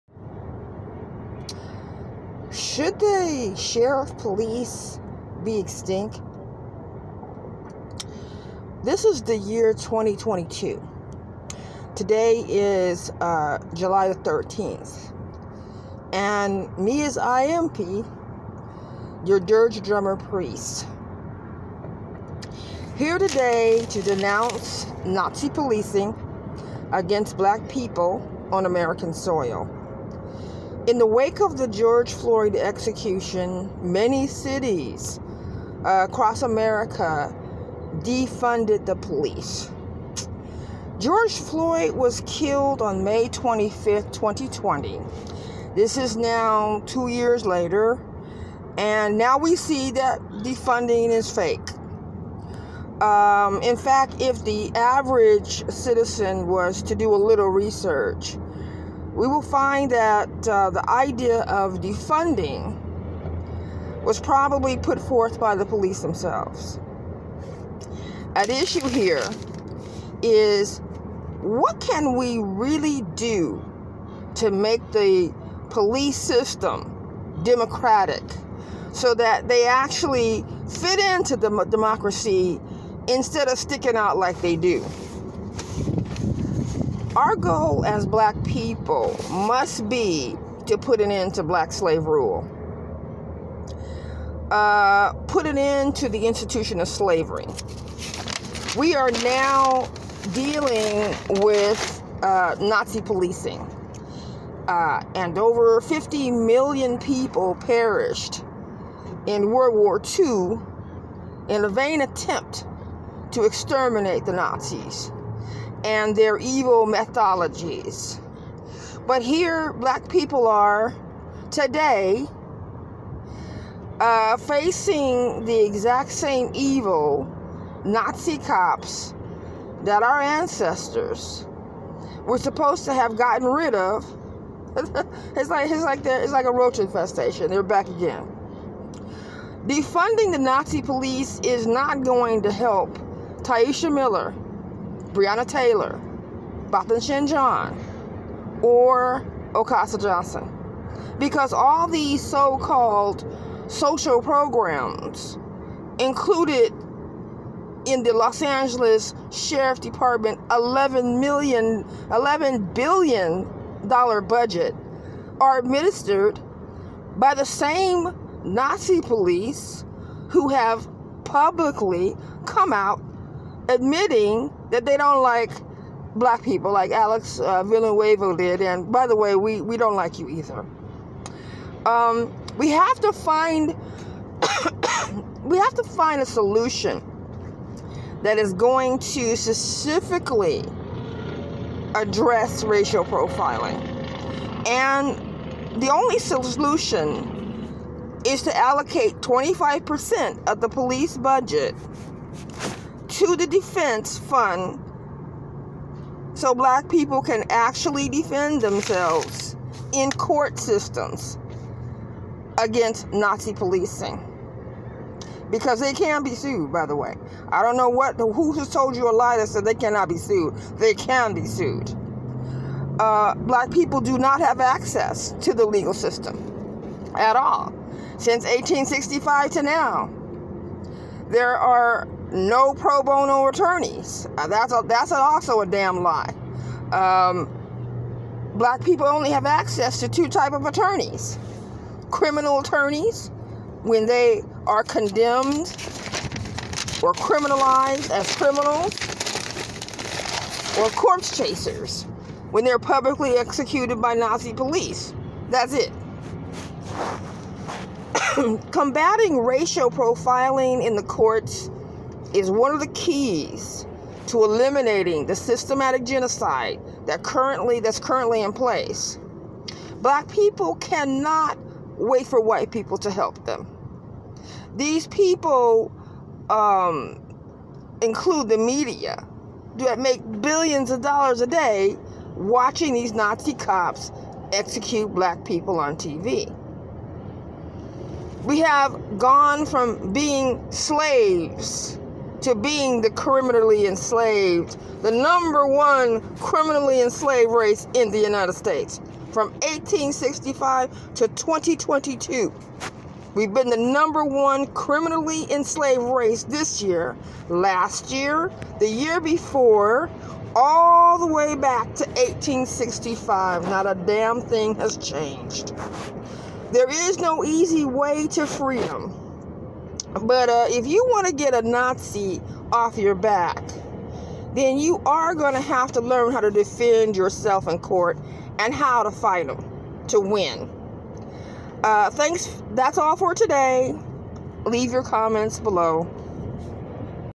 Defunding the police is fake!
Black people need a legal defense fund.  Take 25 percent of every police budget to defend themselves against racial profiling.